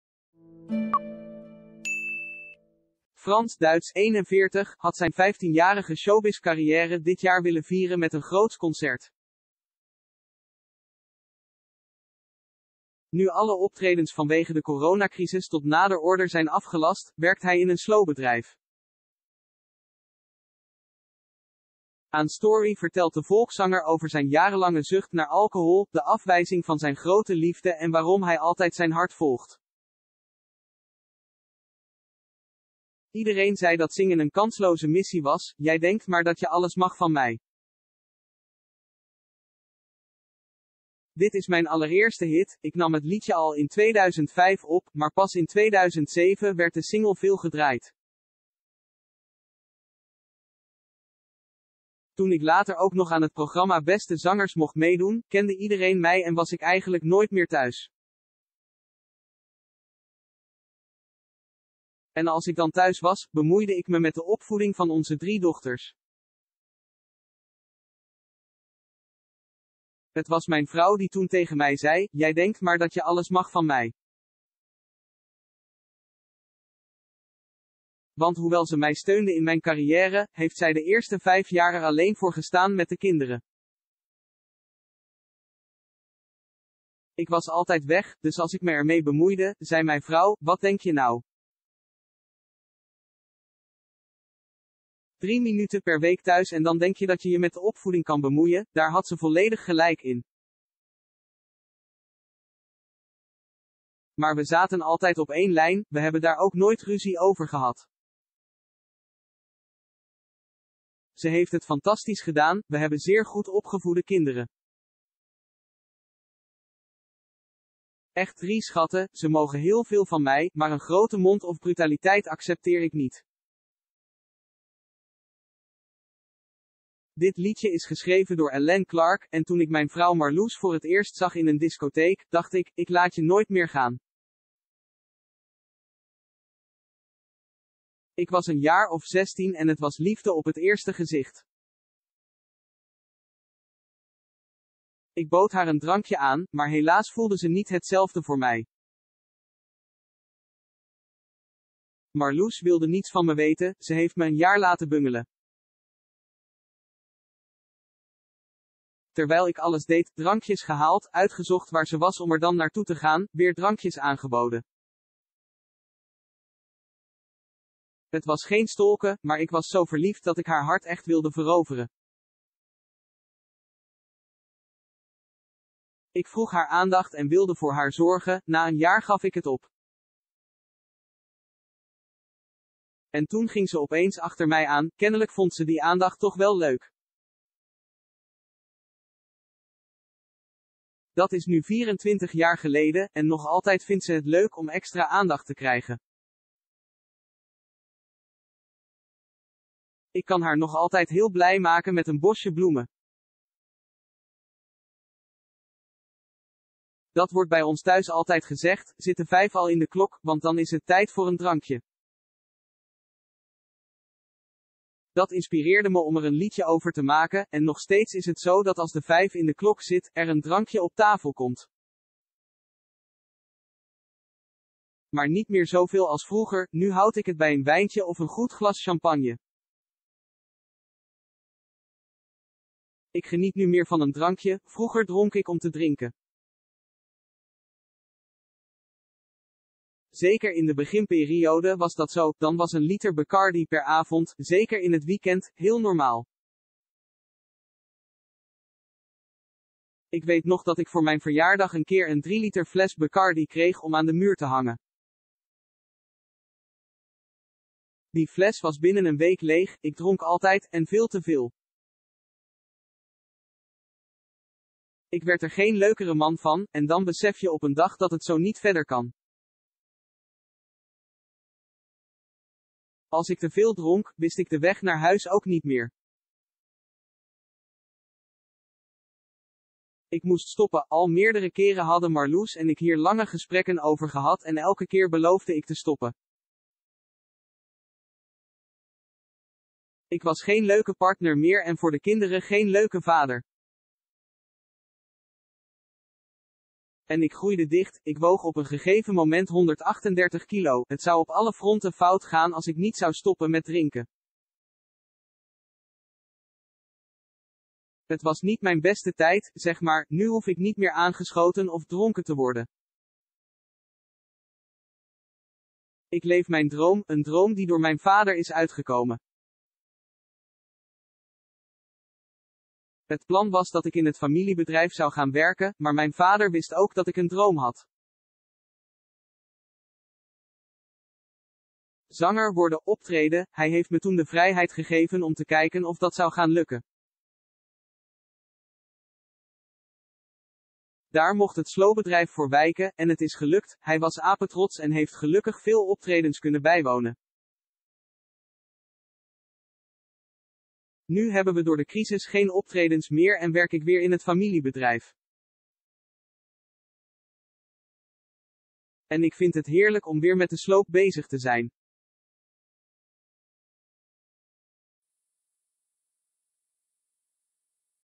Frans Duijts ‘IK WERD ER GEEN LEUKERE PARTNER EN VADER VAN’
NL- Entertainment| Frans Duijts  ‘IK WERD ER GEEN LEUKERE PARTNER EN VADER VAN’
Frans Duijts (41) had zijn vijftienjarige showbizzcarrière dit jaar willen vieren met een groots concert.
Nu alle optredens vanwege de coronacrisis tot nader order zijn afgelast, werkt hij in een sloopbedrijf. Aan Story vertelt de volkszanger over zijn jarenlange zucht naar alcohol, de afwijzing van zijn grote liefde en waarom hij altijd zijn hart volgt. ‘Iedereen zei dat zingen een kansloze missie was...’
Bron: